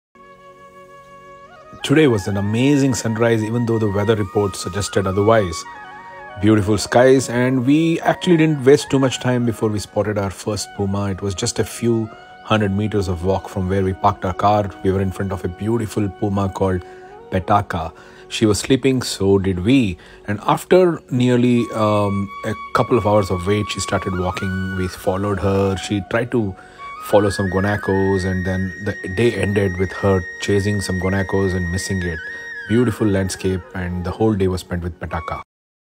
Patagonia Puma Expedition - Day 02
A fabulous day 2 in torresdelpaine patagonia on the toehold.photo - Puma Paradise Expedition